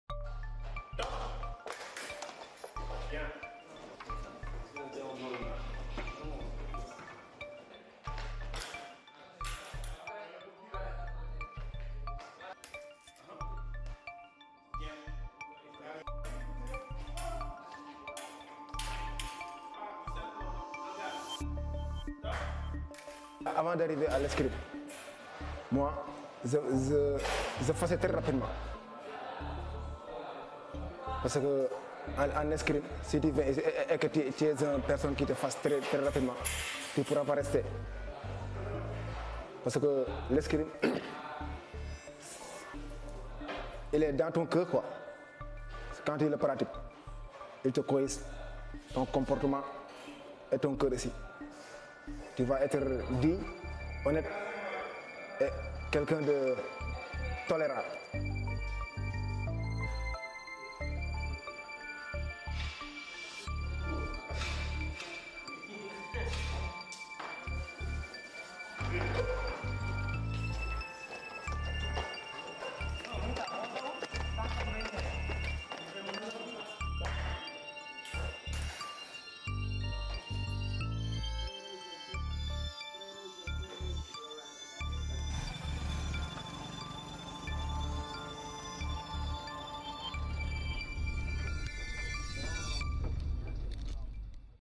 The Senegal jail teaching teenagers to use swords
Weekly fencing training is taking place at Thies jail as part of an experiment to reinsert young offenders into society.